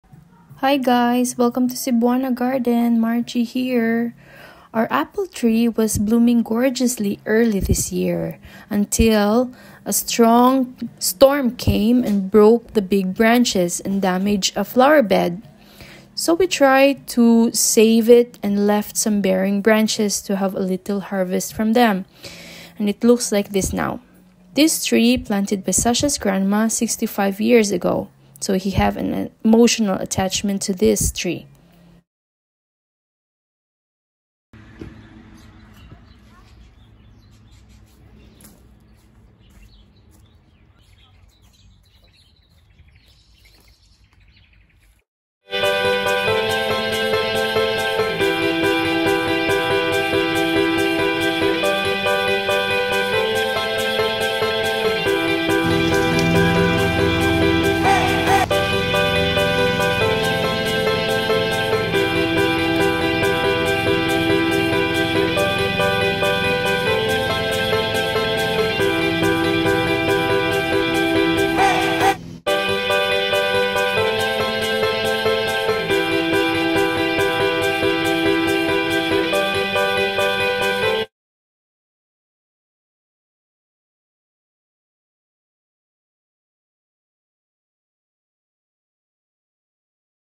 Saving Grandma's Apple Tree
Hi friends, sharing you a Video on saving our 65 yr. old Apple Tree, this tree have witnessed the transformation of this place, so we kind of made this as Landmark and part of the Family heirs.We made a  lot of Chutneys, Compote, and Apple Juice from this plant. So its hard to say goodbye.
Come and join me, see you around.